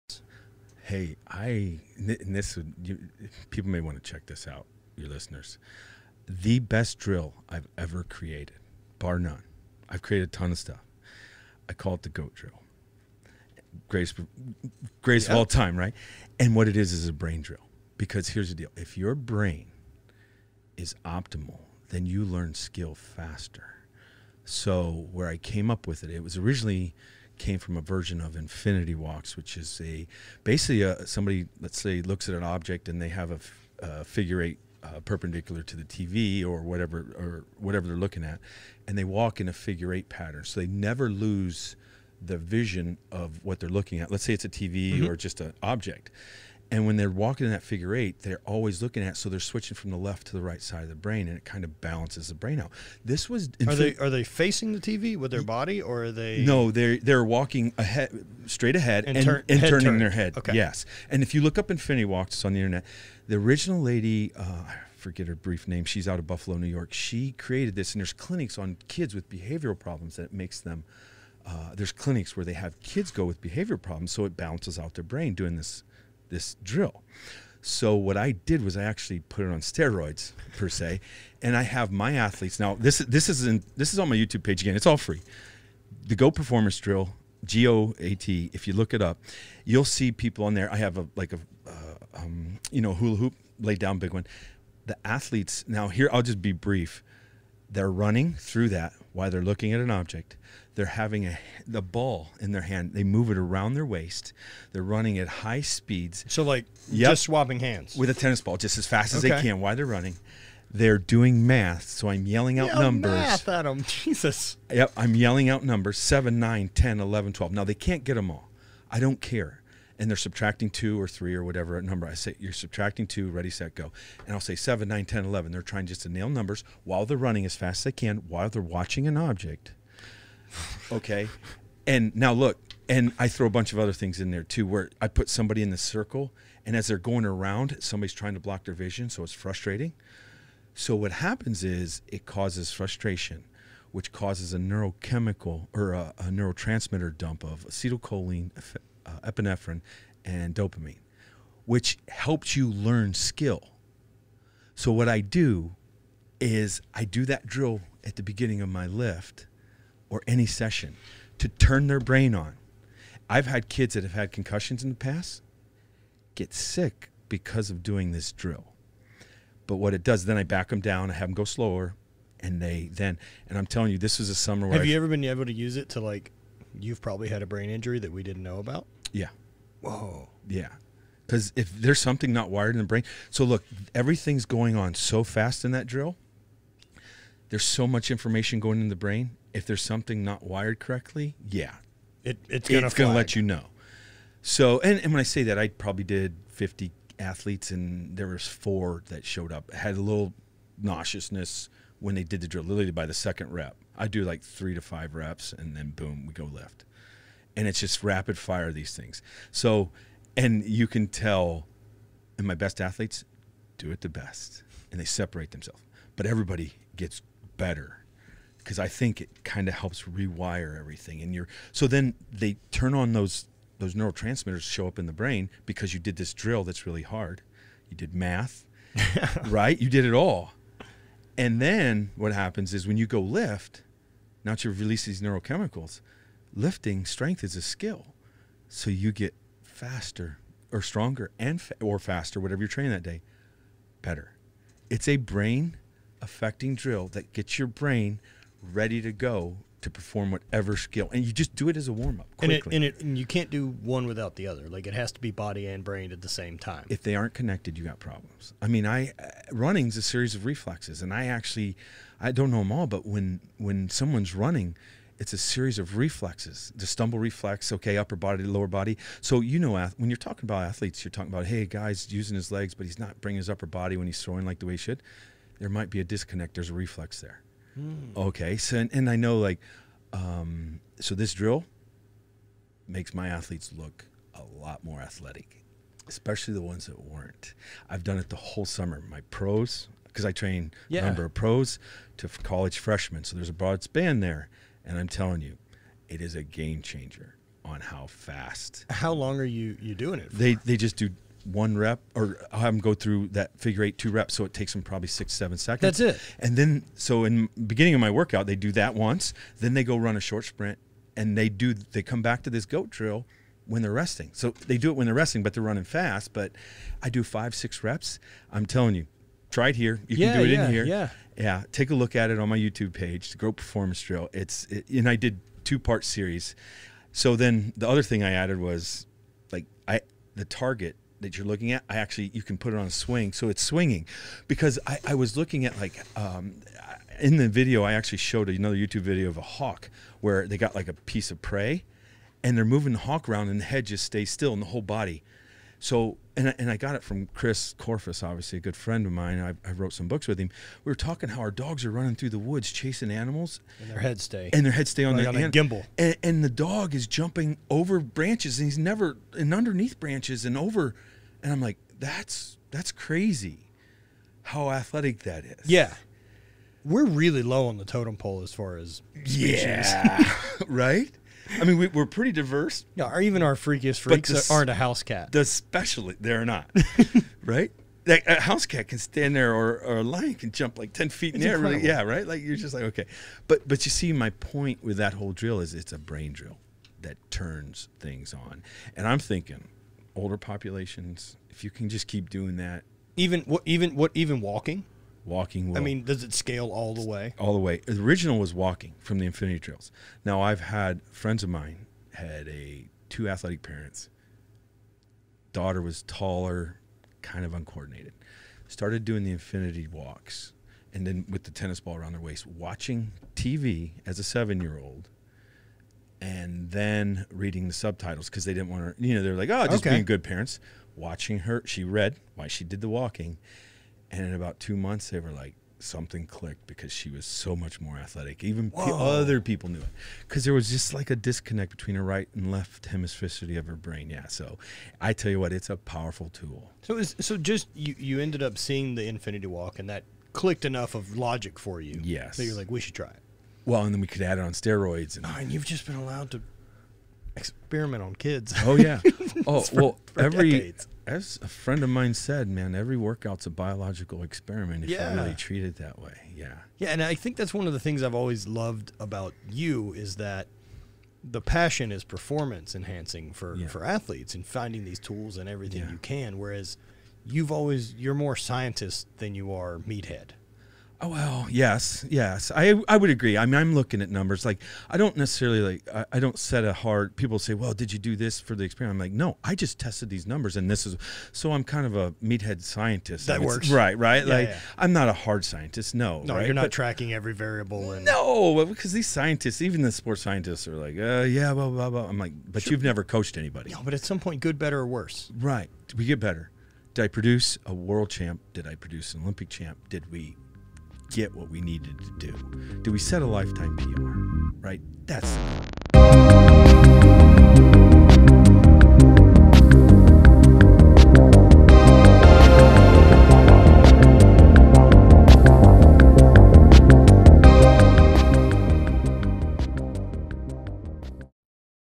UMSO PODCAST 193 - CAL DIETZ - ON CREATING THE GOAT DRILL
Cal Dietz

Excited to have Cal in this week and we had a great chat. We talk a lot about movement and the training regiments Cal has put into place for his athletes and many others to optimize performance. Cal is incredibly intelligent and good at what he does. He has a ton of good advice on how to improve yourself as a human being which I highly recommend you listen to. Okay, let's get to it!

Caveman Coffee

Hybrid Performance Method

Daily Destroyer - team training and programming